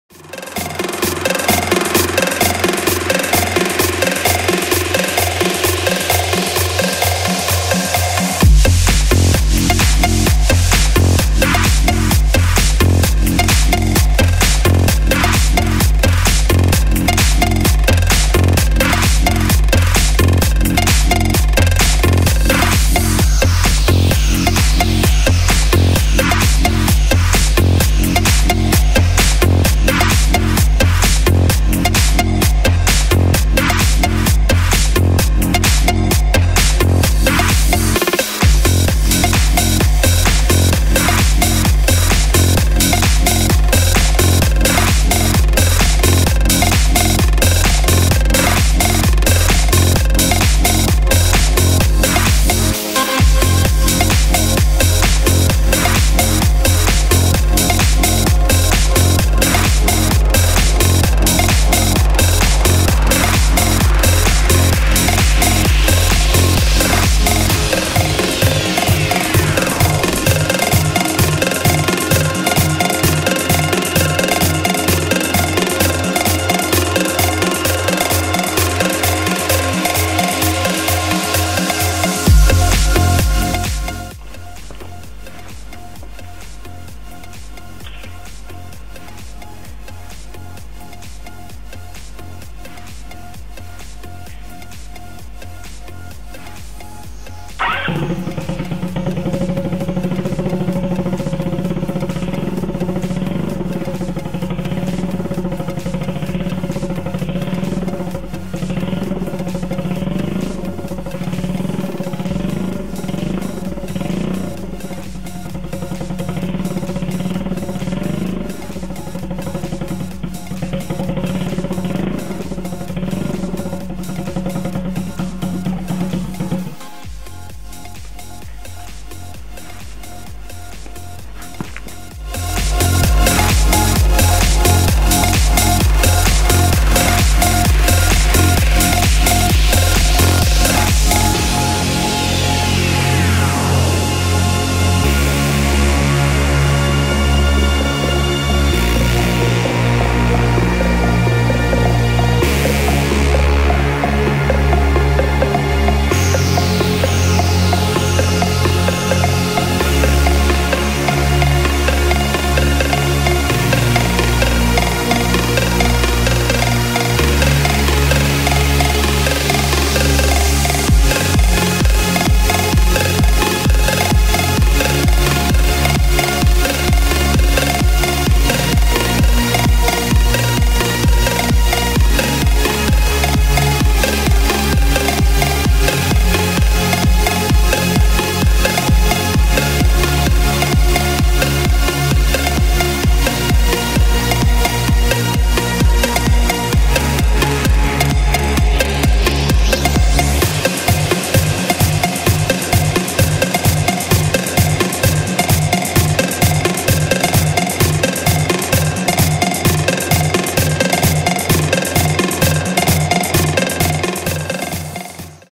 Custom tubular frame ruckus style scooter with GY6 build/fab in progress
Just figured out the wiring and wanted to shoot a little video to celebrate the first time the 150cc GY6 motor got fired up.